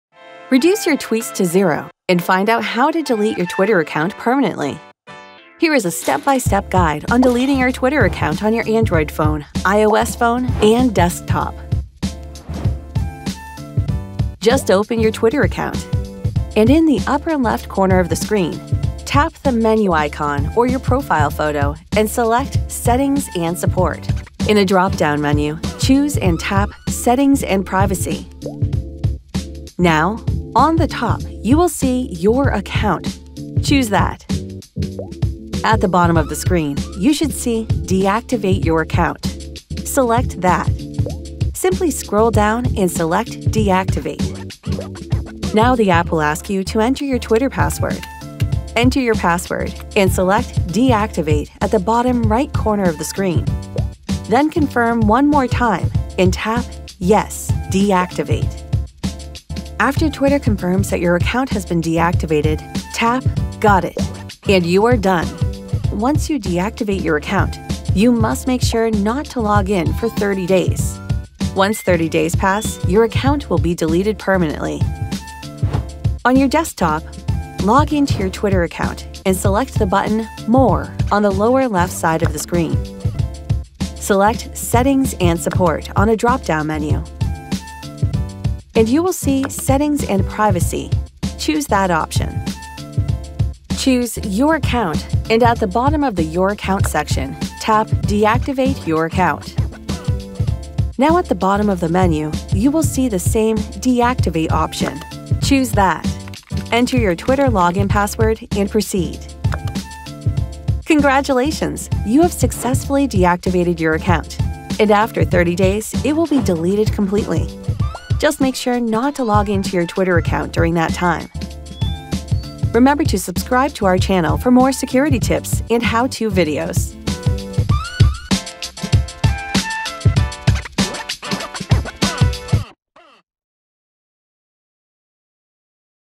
Delete your Twitter account PERMANENTLY: A step-by-step guide
How to delete your Twitter account fast and easy👌
🚀 Improve your online privacy with NordVPN 🔒

If you are wondering how to delete your Twitter account permanently, follow this easy step-by-step guide and learn how to delete your Twitter account on your Android phone, iOS phone, or desktop.

► RECOMMENDED VIDEOS ►
How to delete your Facebook account permanently:

Contents:

00:00 Intro
00:13 How to delete your Twitter account on an Android phone
00:13 How to delete your Twitter account on an iOS phone
01:17 How to delete your Twitter account on desktop

How to delete your Twitter account on an Android phone

1. Just open your Twitter account, and in the upper left corner of the screen, tap the menu icon or your profile photo and select "Settings and support."
2. In a dropdown menu, choose and tap "Setting and privacy."
3. Now on the top, you will see "Your account" choose that.
4. At the bottom of the screen, you should see "Deactivate your account" select that.
5. Scroll down and select "Deactivate."
7. Enter your password and select "Deactivate" at the bottom right corner of the screen.
8. Then confirm one more time, and tap "Yes, deactivate."

 How to delete your Twitter account on an iOS phone

1. Just open your Twitter account and in the upper left corner of the screen, tap the menu icon or your profile photo and select "Settings and support."
2. In a dropdown menu, choose and tap "Setting and privacy."
3. Select "Your account."
4. At the bottom of the screen, tap "Deactivate your account."
5. Scroll down and select "Deactivate."
6. Enter your password and select "Deactivate" at the bottom right corner of the screen.
7. Then confirm one more time, and tap "Yes, deactivate."

How to delete your Twitter account on your desktop

1. Log in to your Twitter account and select the button "More" on the lower left side of the screen.
2. Select "Settings and support" on a drop-down menu, and you will see "Settings and privacy" choose that option.
3. Select "Your account," and at the bottom of the "Your account" section, tap "Deactivate your account."
4.  Now, at the bottom of the menu, you will see the same "Deactivate" option; choose that.
5. Enter your Twitter login password and proceed. Congratulations, you have successfully deactivated your account, and after thirty days, it will be deleted completely.